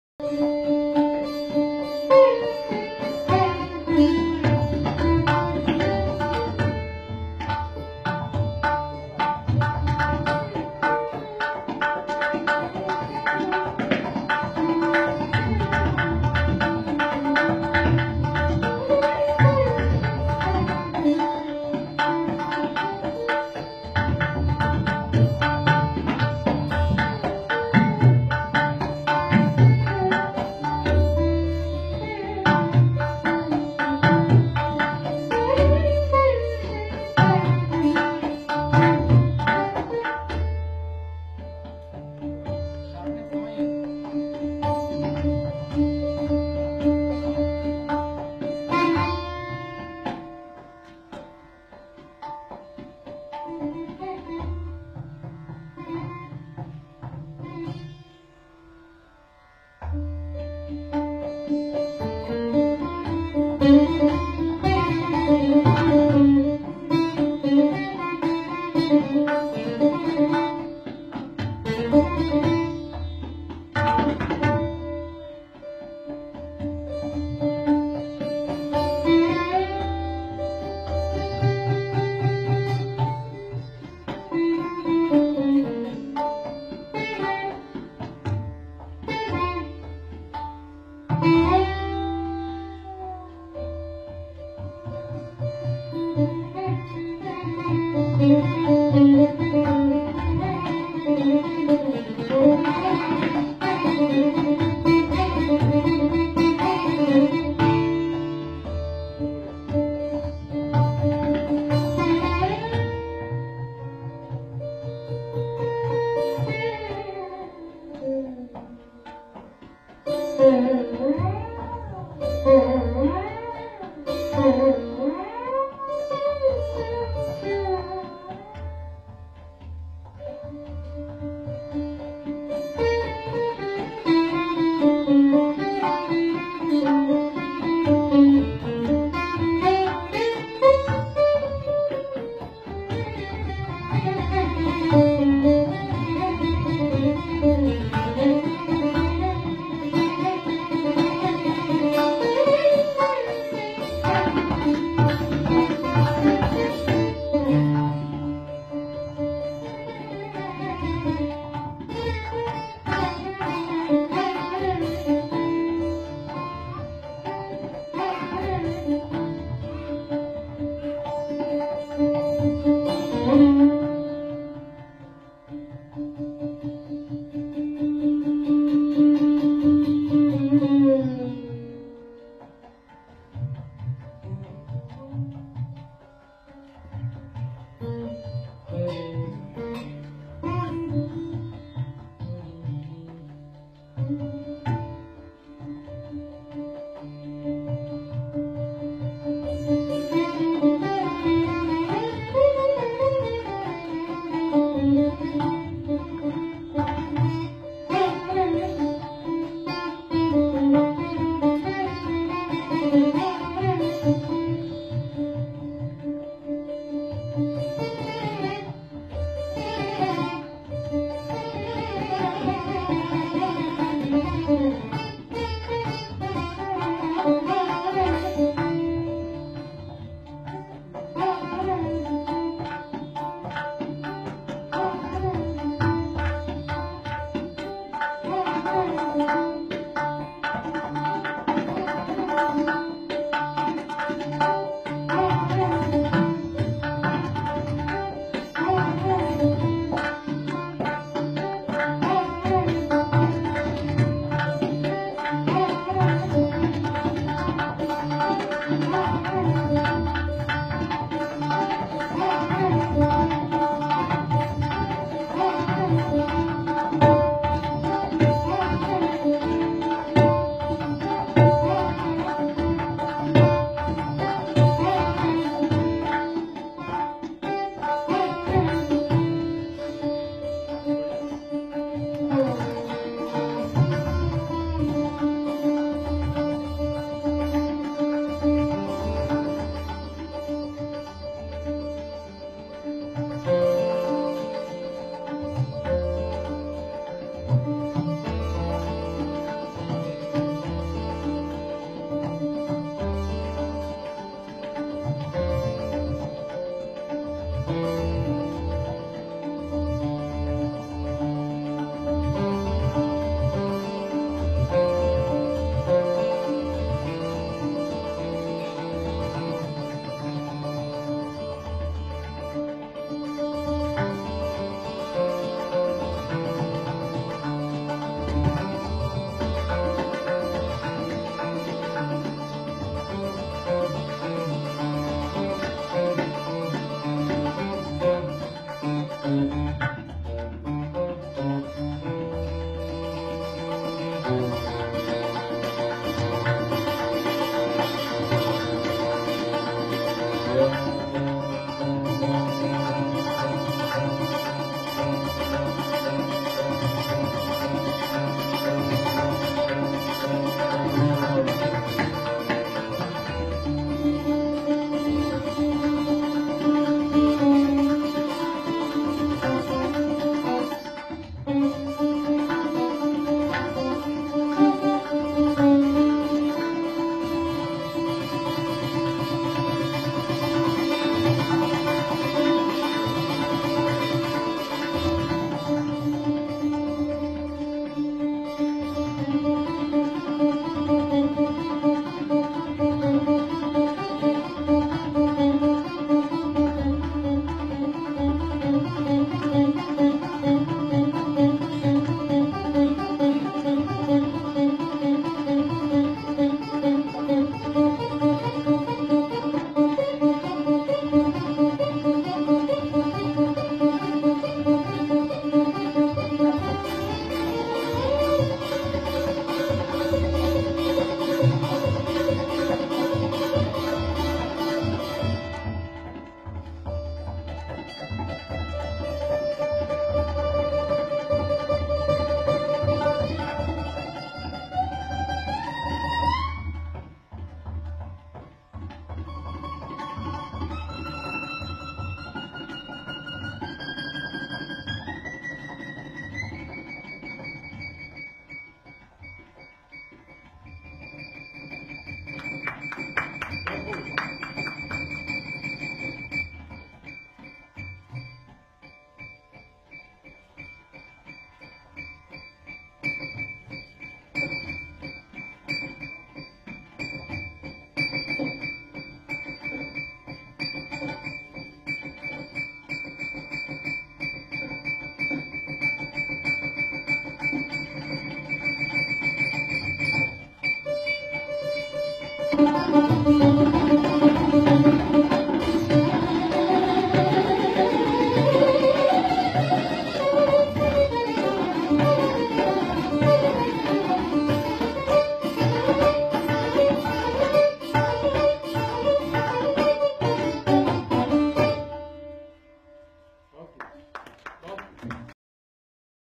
Siddha Veena & Tabla I Siddhartha Banerjee & Sourabh Goho I Live in New Delhi
Habitat World presents "Strings & Rhythm"

Siddha Veena: Siddhartha Banerjee
Tabla: Sourabh Goho

Raaga: Kirwaani in Teental (16 beats)
Glimpses from Vilambit, Drut and Jhala.

Date: Wednesday, 19th April 2017
Venue: India Habitat Centre, Delhi

Video Courtesy: Yash Sagar